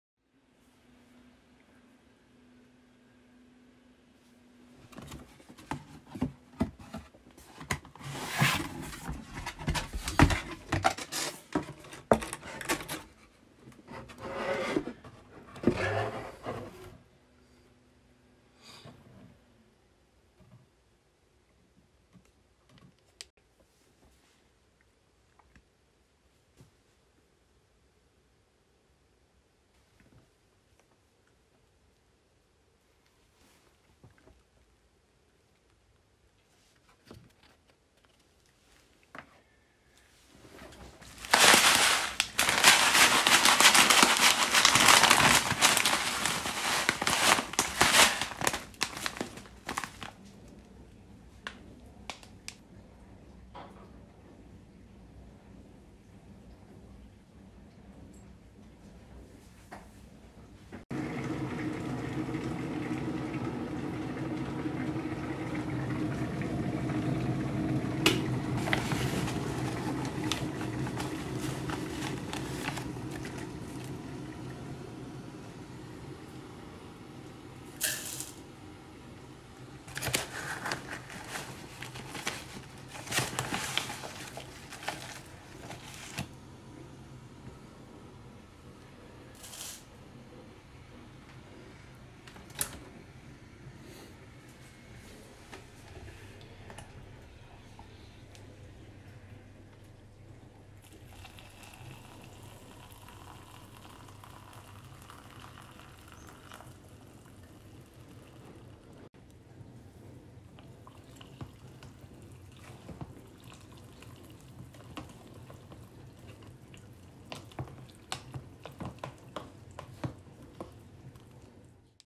Huel Hot And Savoury Unboxing
Unboxing and cooking instructions for Madras, Mexican Chili and Thai Green Curry flavours